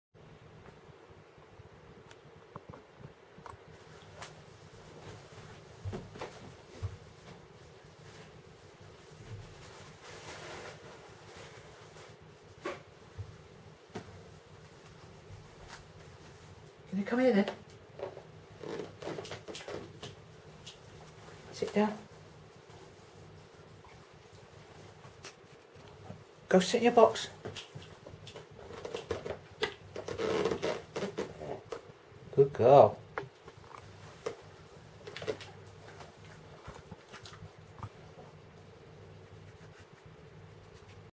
Dog Obedience
Molly goes to and from her box dog obedience

Molly is about 5 years old.  She was about 31/2 years old when I got her from the Local Dog and Cat Home.  She is a Staffordshire Bull Terrier Cross (staffie). I have no idea what she is crossed with, but that is what they told me at the home.  She had little training when she first came to me but she knew sit and was well house trained.  It didn't take her long to learn her new name and now she knows about 20 different commands and a few tricks.  Reward based training and a fair bit of patience is required but the rewards are worth it.  She still is very wary of other dogs and has some issues with recall and separation anxiety but hey, non of us are perfect :)
I had never had a dog before Molly.